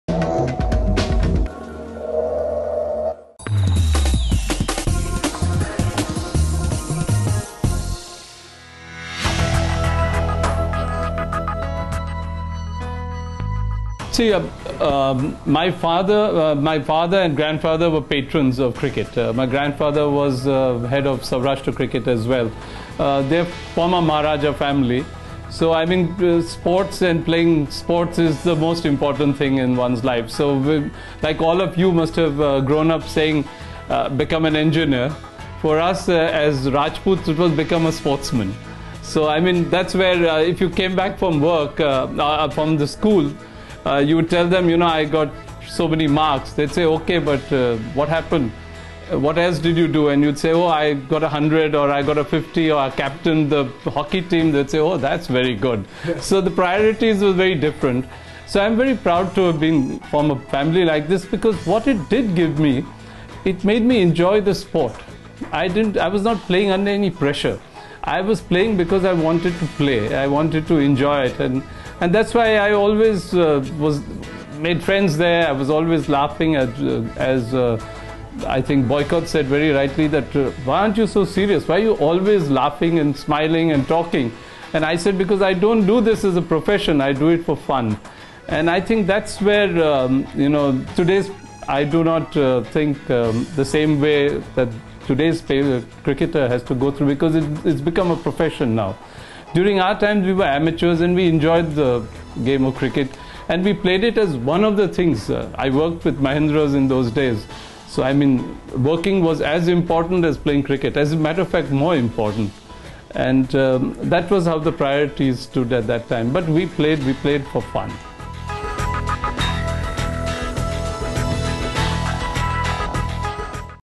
Yajurvindra Singh talks about his family's sporting tradition
Yajurvindra Singh played Test cricket for India in the 1970s and has the distinction of holding two world records on debut.

Yajurvindra comes from a royal family from Saurashtra and it had a rich sporting tradition. He says that as a child, he was encouraged to play sports and it had become a way of life. The family took pride in performances on the sports field and that is how he took up cricket. For him, cricket was a source of enjoyment and he also had a job. As a result, he had to strike a balance between the two during his playing days. This tells you the kind of tradition a royal family used to have for sports.